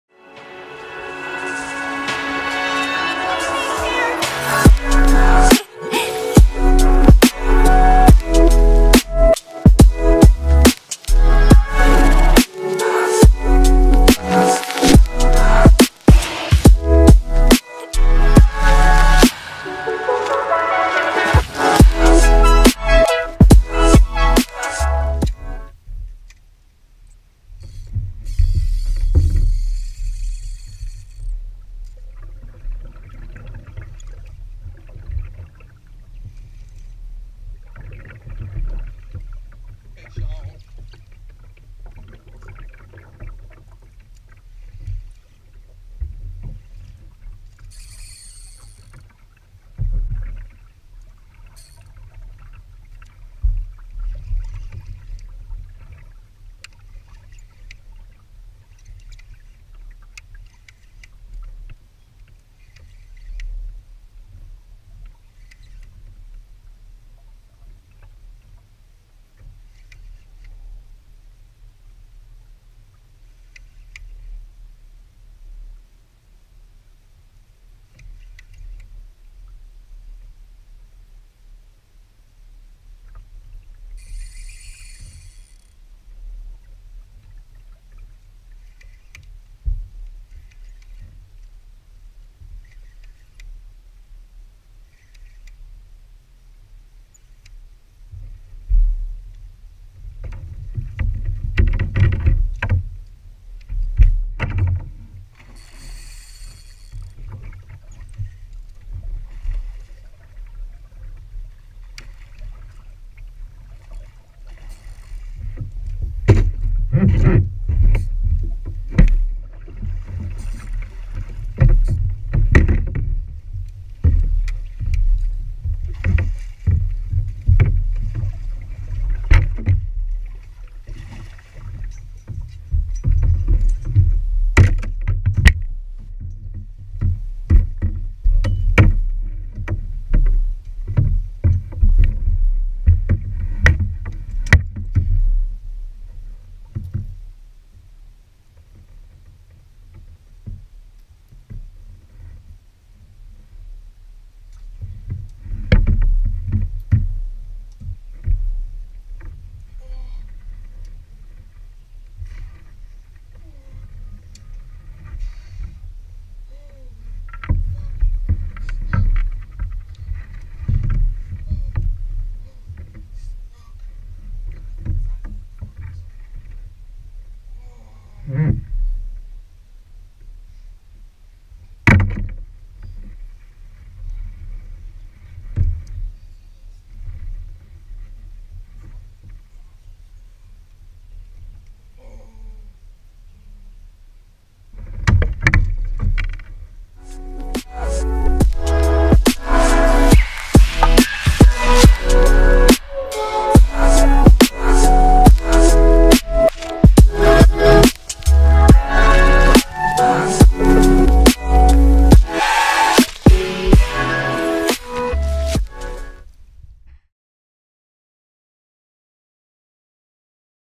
Kayak Fishing for Monster Snook - Snook Fishing on Native Ultimate 12
Just got a new kayak the Native Ultimate 12, took it for a quick morning paddle and it turned out to be an epic trip. Got hooked up to this giant snook on light tackle. After a couple minutes battling it through docks and pilings, I was able to finally land him.

Follow Social media -   Instagram @ReelFinsFlorida